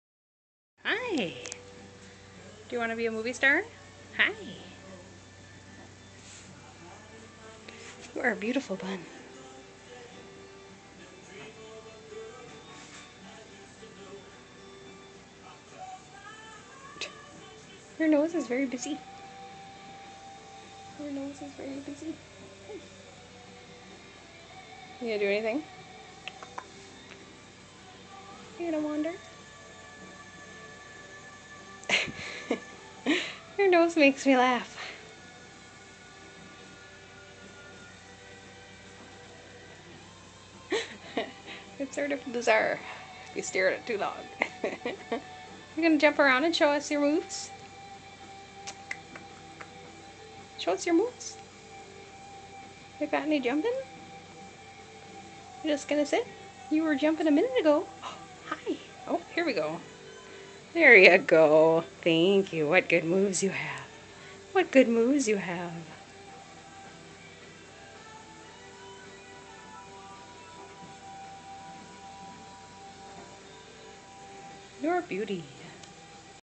Basil -- Adopted!!
Basil is a rabbit adopted from Prairie's Edge Humane Society in Faribault, MN.